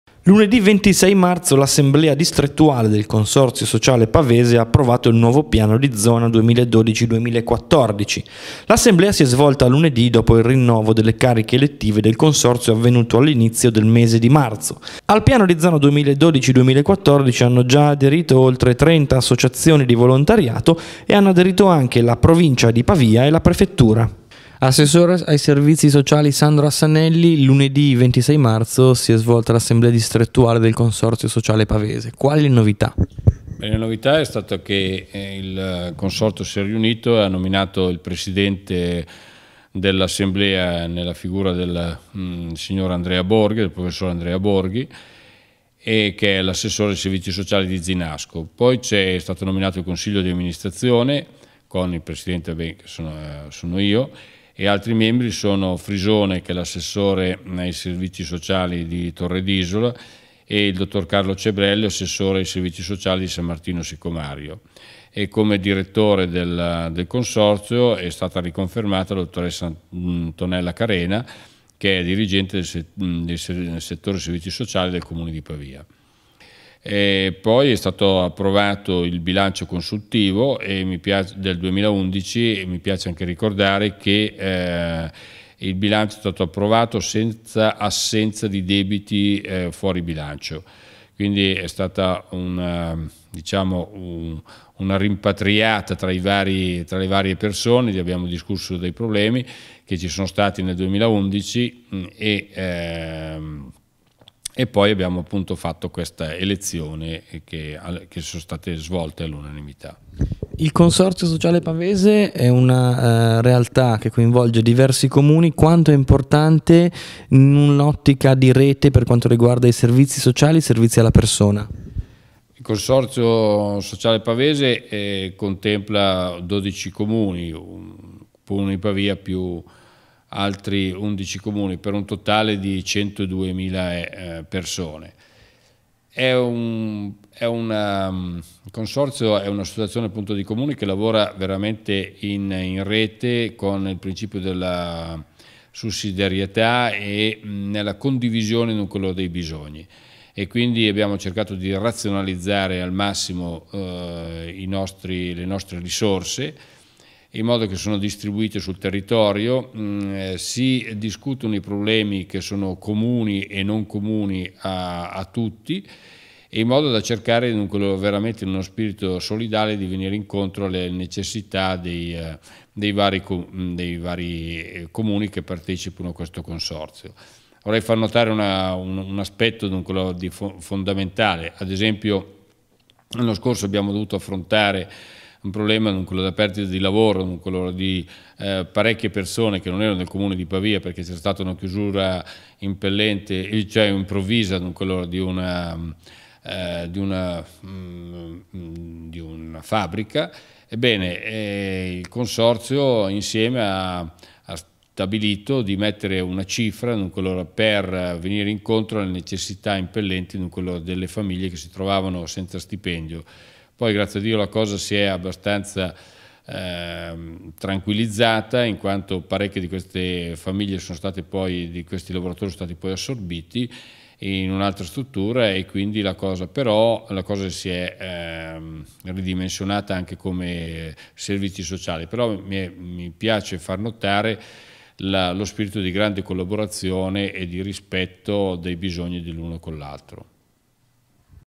Piano di Zona 2012-2014
Il Consorzio Sociale Pavese approva il piano di Zona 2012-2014 ne parla l' Assessore ai Servizi Sociali Sandro Assanelli